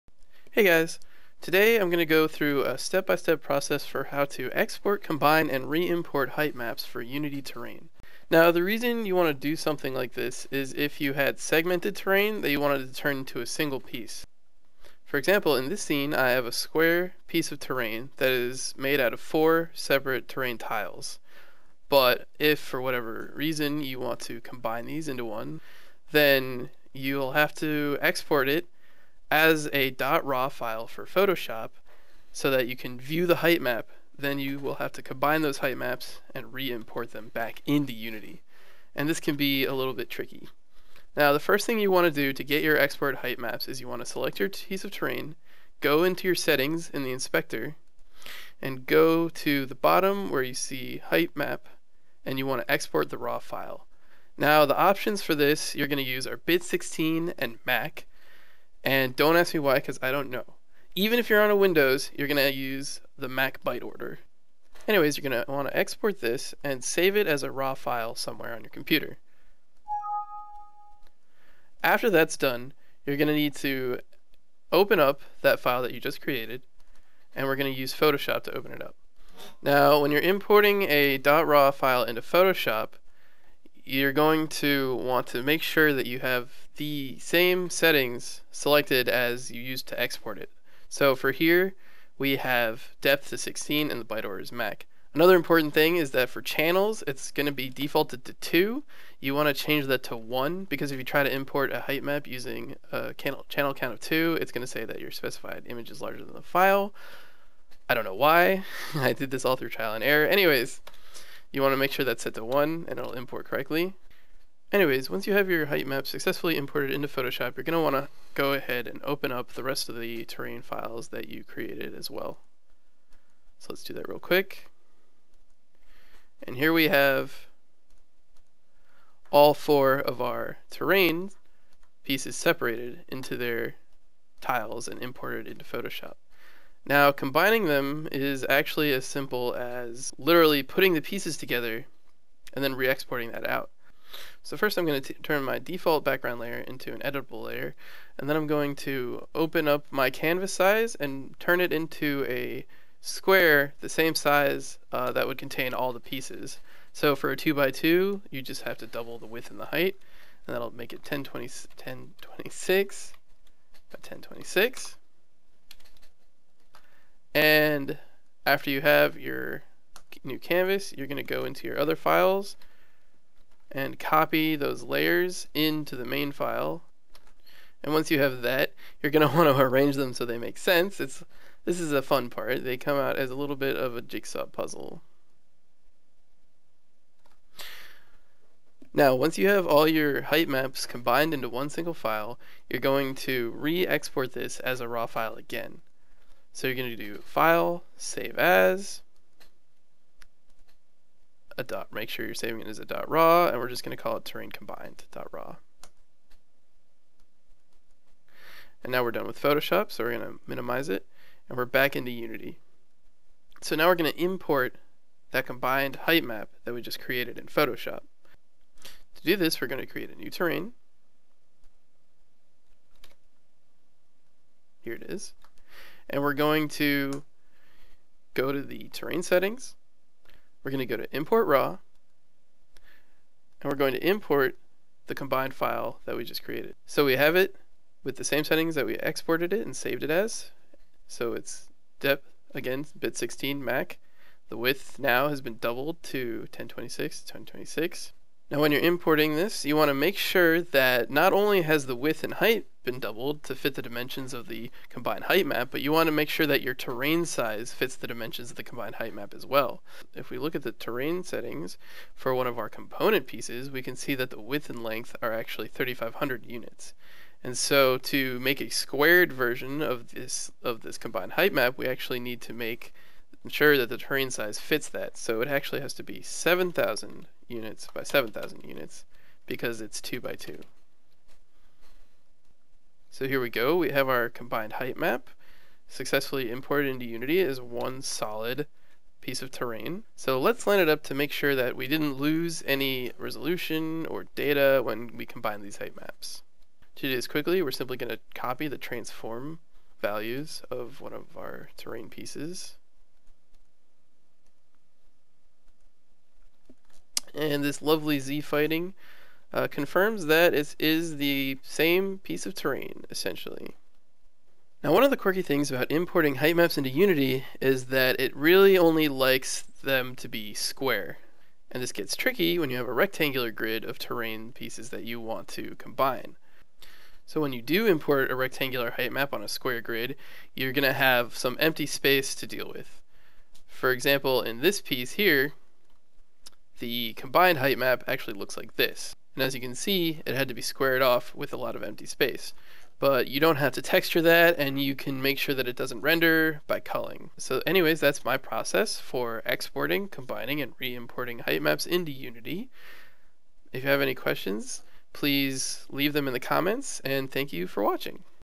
UNITY TUTORIAL: Exporting, Combining and Re-Importing Unity Heightmaps
Here's my step-by-step process for exporting, combining, and re-importing Unity terrain height maps. This is my first tutorial so please leave questions and feedback in the comments!

PS I say anyways a lot :P